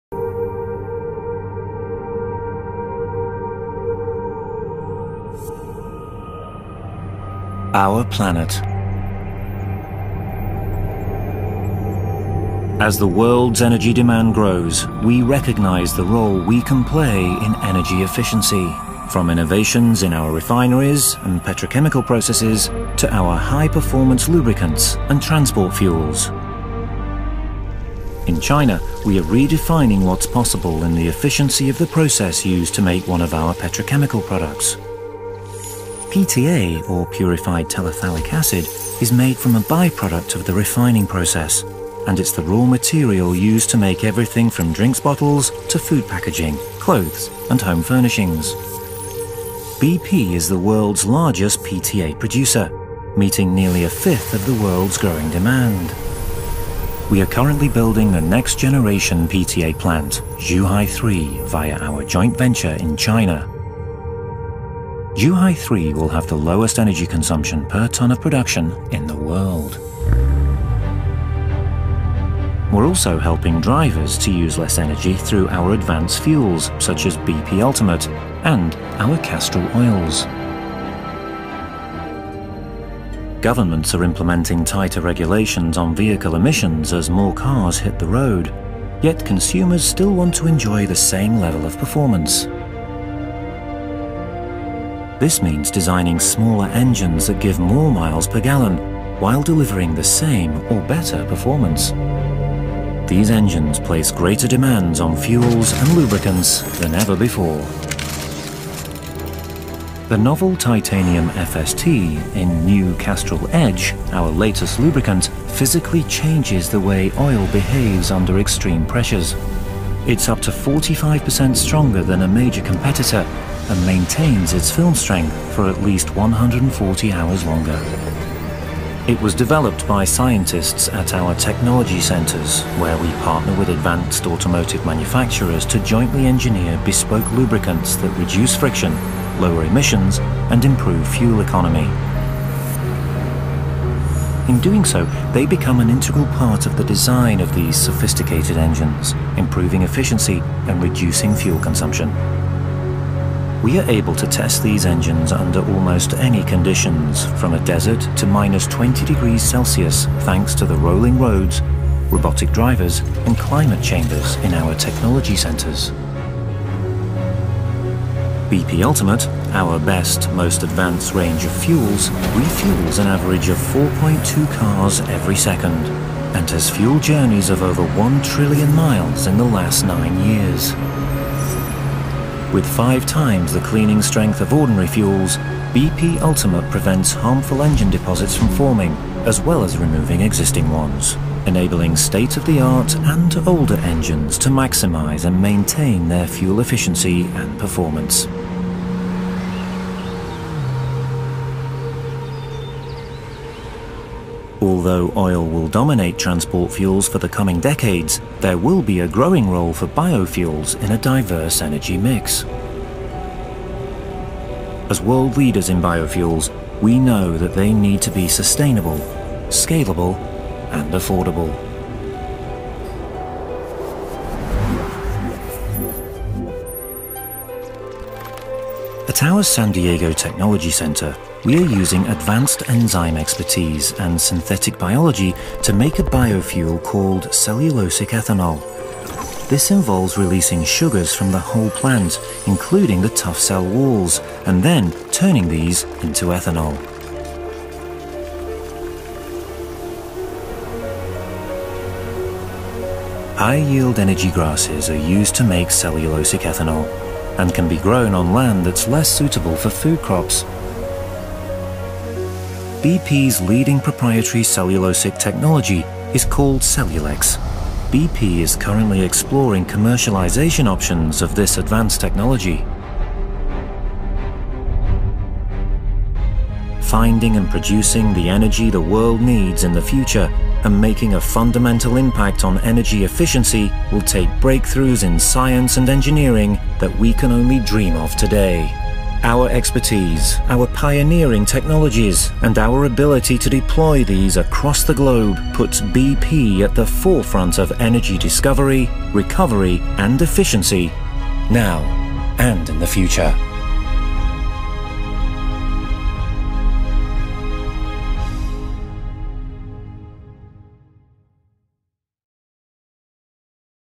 More Efficiency
See how technologies for today, and tomorrow, are helping to make the most of every drop of oil that we produce.